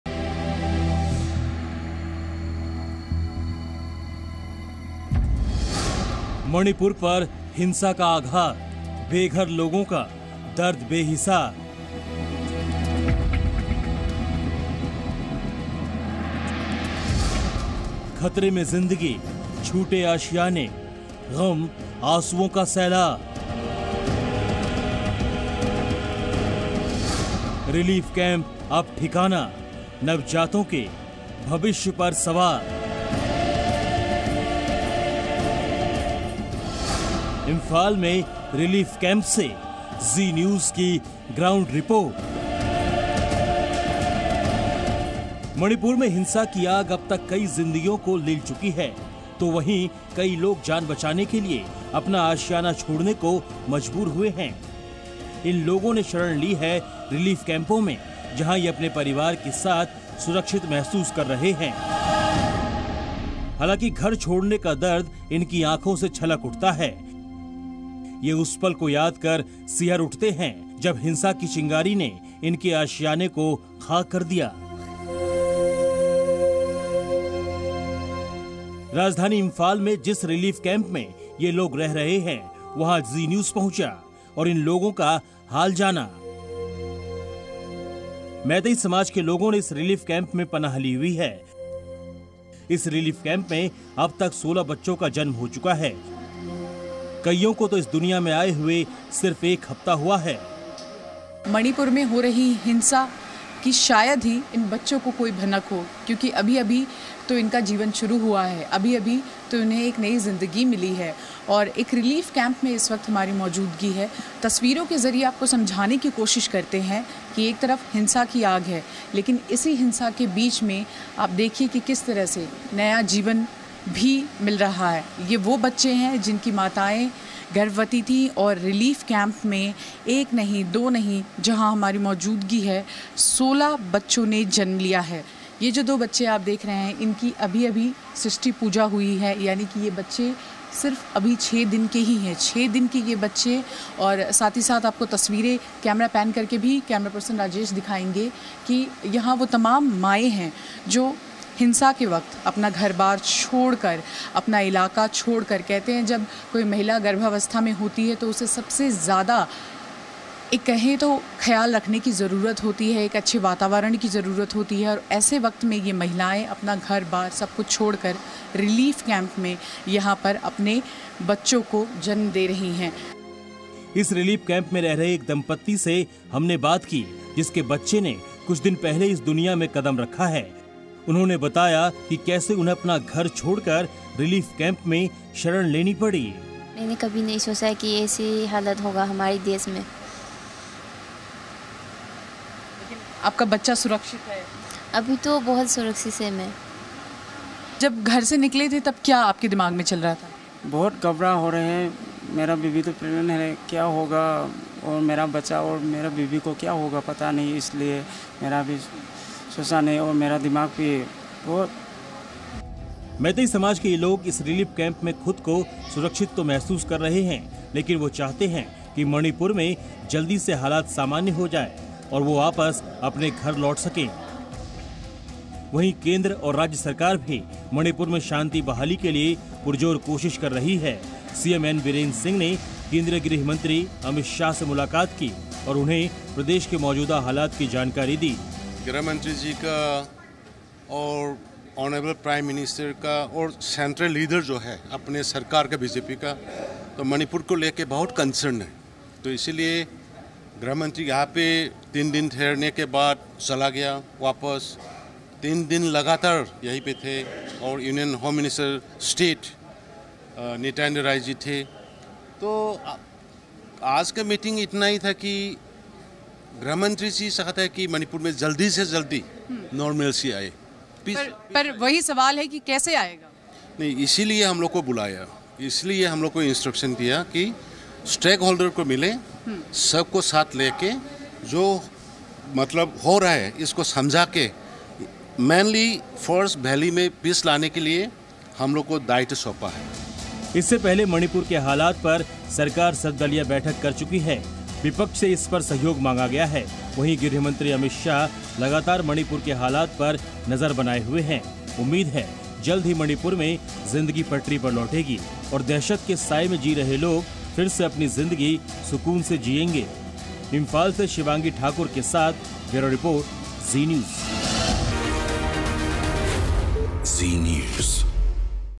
Manipur में आंसुओं का सैलाब...हिंसा के बीच Relief Camp में 16 बच्चों का जन्म | ZEE NEWS Report
Manipur Violence News: मणिपुर में गम और आंसुओं का सैलाब आया हुआ है हिंसा के बीच रिलीफ कैंप में  बच्चों का जन्म हुआ है. देखिए Zee News की ग्राउंड रिपोर्ट

Manipur Violence News: There is an inundation of sorrow and tears in Manipur, amidst the violence, children have been born in the relief camp. Watch the ground report of Zee News